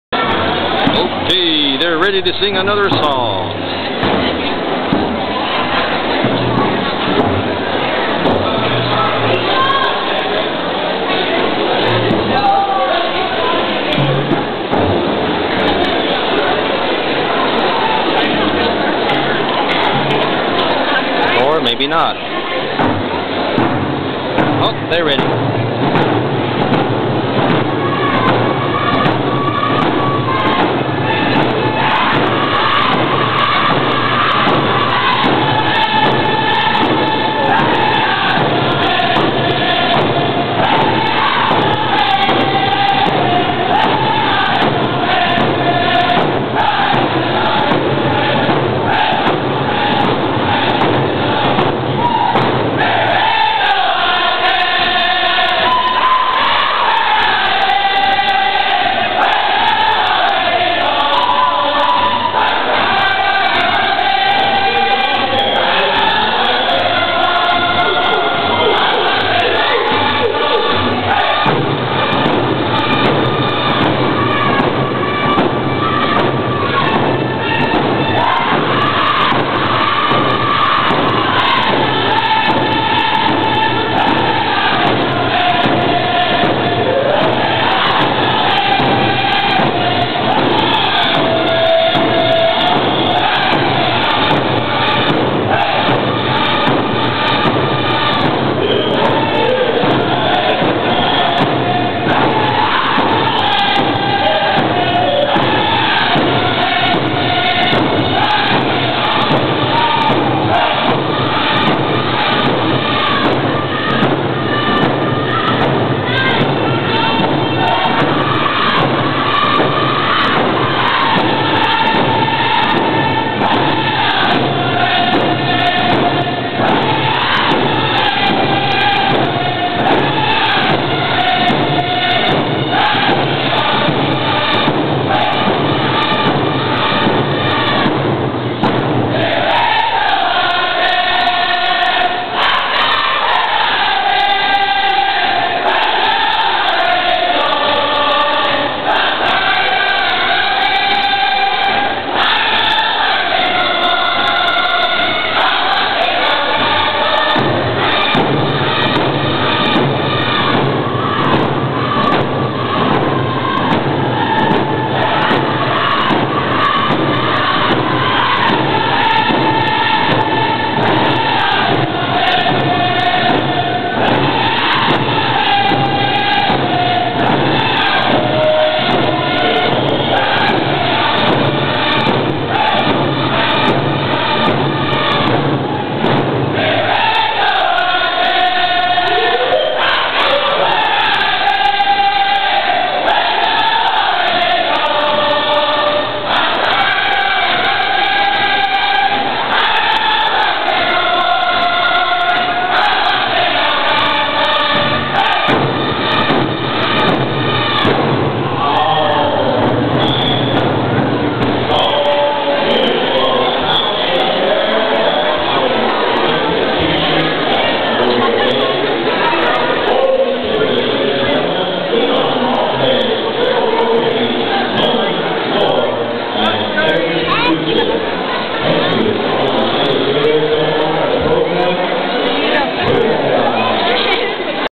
Saskatoon Round Dance 2
More of the same:  this event was held at the White Buffalo Youth Centre on Friday, March 14, 2008.